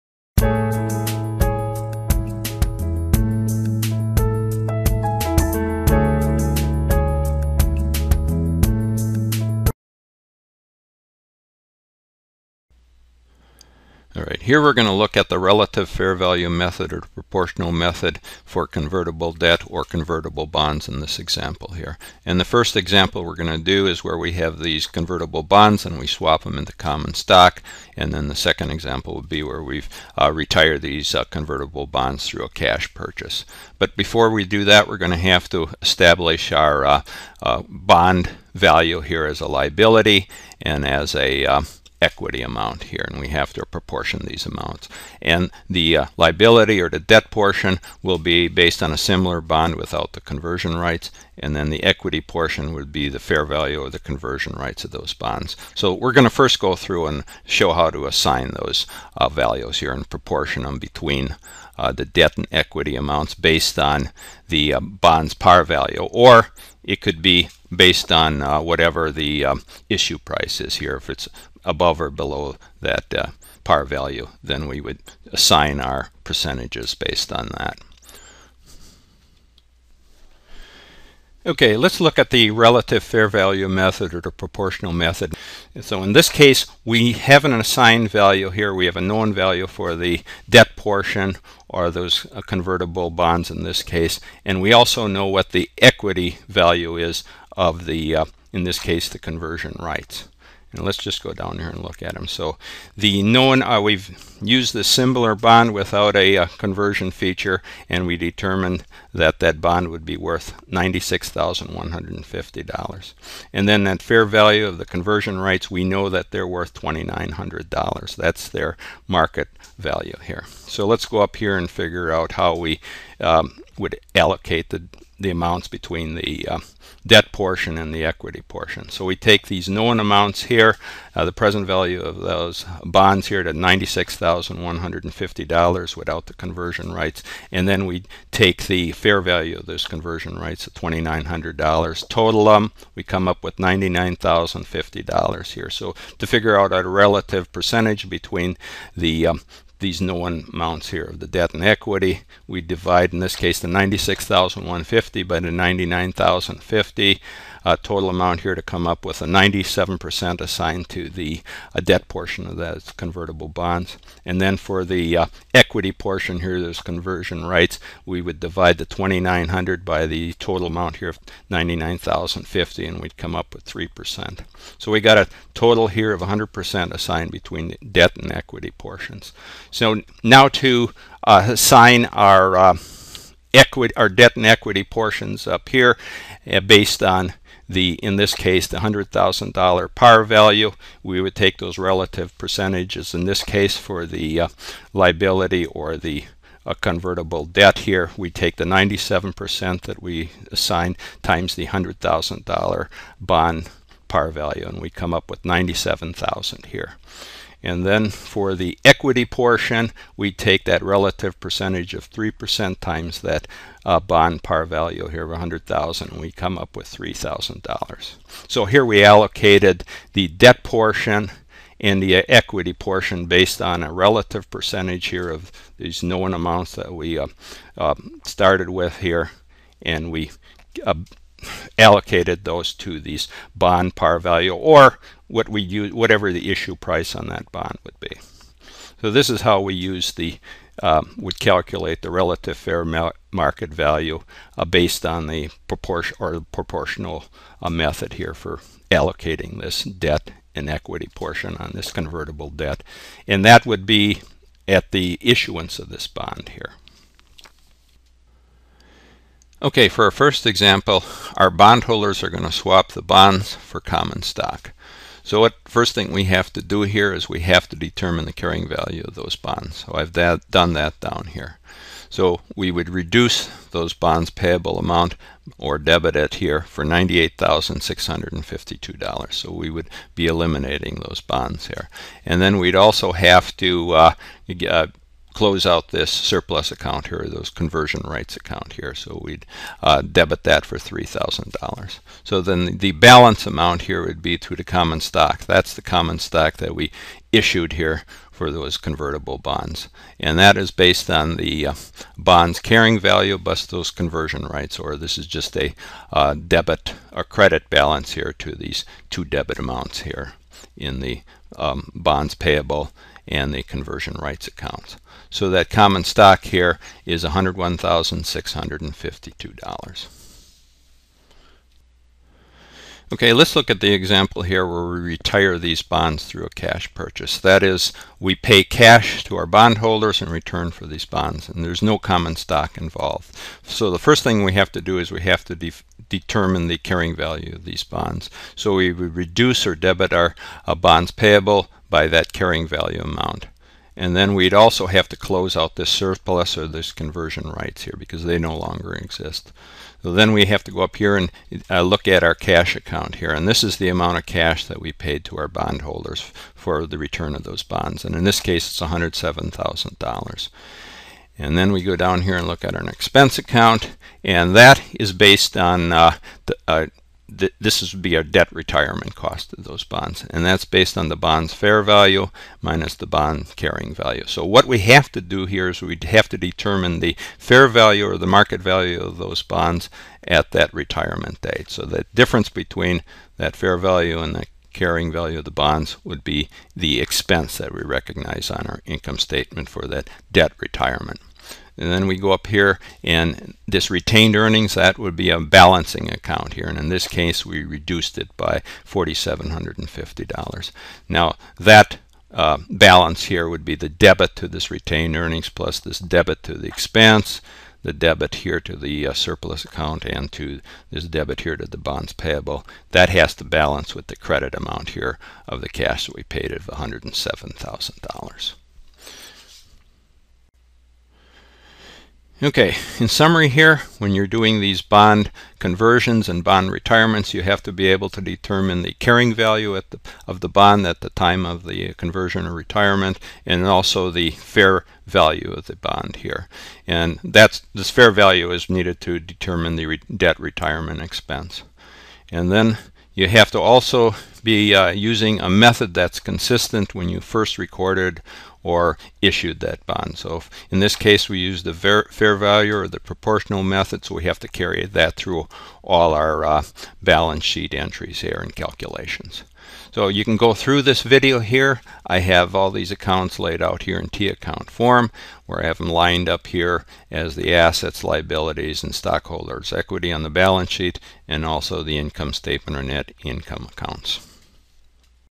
Convertible Bonds Issuing And Retiring Using Relative Fair Value (Proportional) Method
Convertible bonds (debt for equity swap), accounting using the relative fair value (proportional) method for (1) issuing the bonds, (2) converting bonds into common stock and (3) using cash purchase to retire the bonds, originally issued convertible bonds, bondholders are holding the bonds, exchange at least a portion of the bonds for common stock, bondholders return that portion back to the company in exchange for common stock, relative fair value (poroportional) method allocates bond between liability and equity portions based on the relative percentage of the bonds present value and fair value of conversion rights, multiplying the relative percentage for debt and equity times the bonds issue price (either at par, discount value, or premium value, what ever is the case) allocates the debt and equity portion for the bond debt, example includes (1) issuing the bonds, (2) converting bonds into common stock and (3) using cash purchase to retire the bonds, complete calculations for accounting required for the bond for the situations listed above, complete balance sheet journal entries (T accounts) shown on balance sheet template, accounting detailed and calculations required explained by Allen Mursau